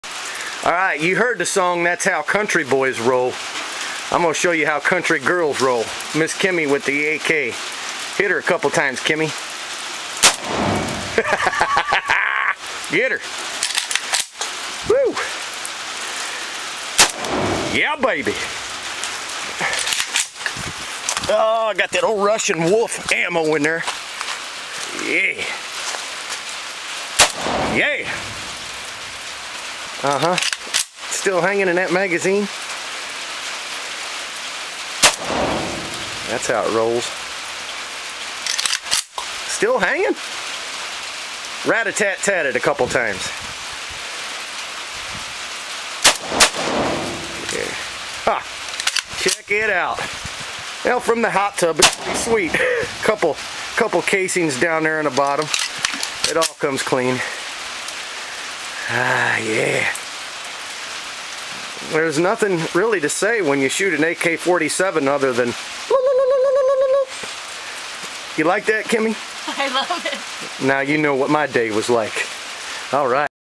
AK 47's and Hot Tubs are a staple in country living. PLEASE SUBSCRIBE!!
Miss Kimmie shoots the AK47 from our Hot Tub here at Honky Tonk Manor. I aint ever satisfied, so I'm always looking for new ways to have fun and this, as simple as it is, brings a great deal of pleasure. As you watch Kimmie fire the AK, I'll tell you that we are trying to burn up this worthless Russian WOLF ammunition that doesn't cycle through the AK properly. Notice that Kimmie is in the habit of cycling the bolt even if the round doesn't stick. You get what you pay for in ammo, and I rarely have any trouble running anything, including reloads threw the AK. Wolf ammo is JUNK.